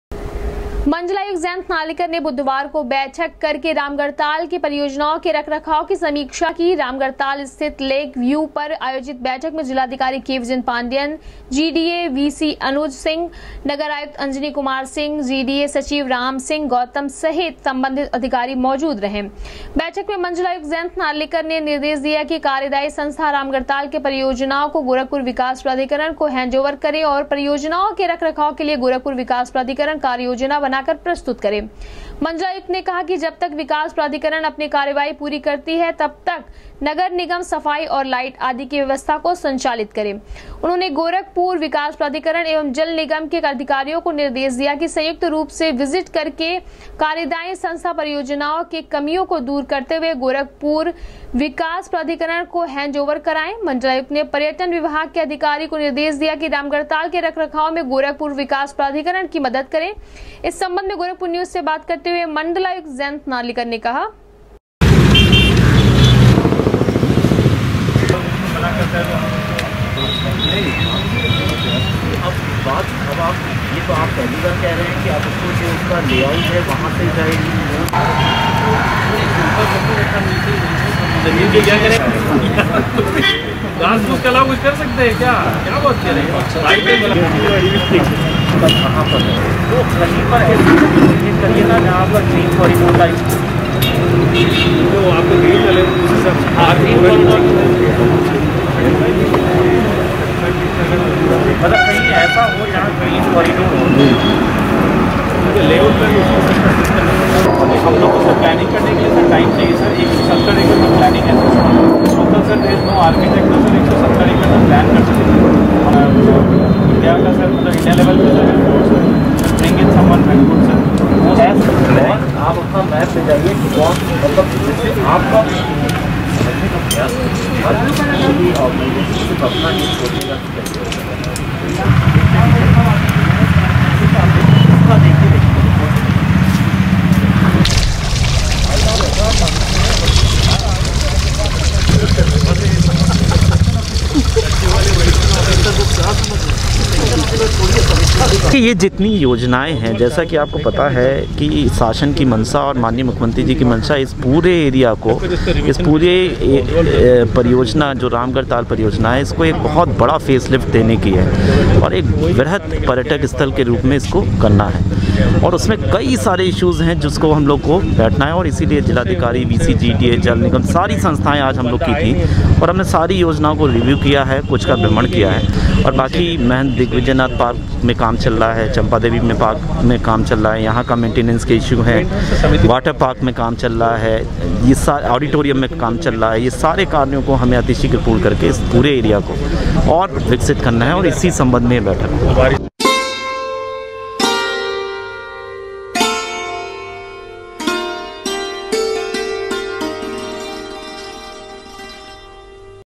मण्डलायुक्त जयंत नार्लीकर की अध्यक्षता में बुधवार को सम्पन्न हुई रामगढ़ताल के परियोजनाओ के रखरखाव की
मण्डलायुक्त जयंत नार्लीकर की अध्यक्षता में बुधवार को सम्पन्न हुई रामगढ़ताल के परियोजनाओ के रखरखाव की समीक्षा बैठक,कार्यदायी संस्था से रामगढ़ताल के परियोजनाओ को गोरखपुर विकास प्राधिकरण को हैंड ओवर करने के दिये निर्देश